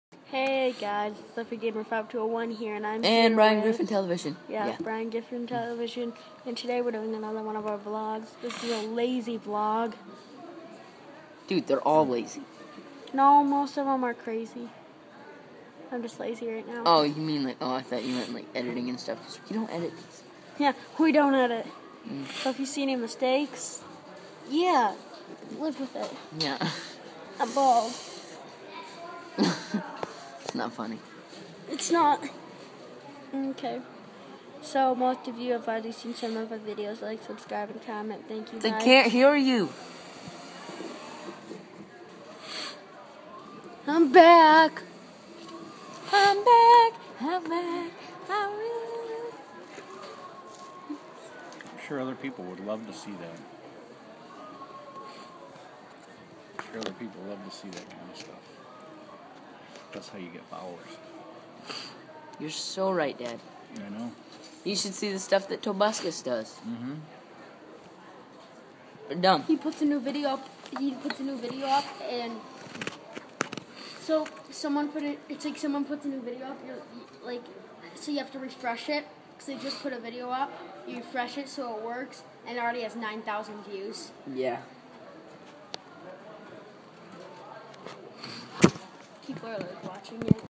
Demmer Center (Vlog #4)
Today we were at MSU's Demmer Center for the JOAD program and we thought it might be a good time to do a vlog.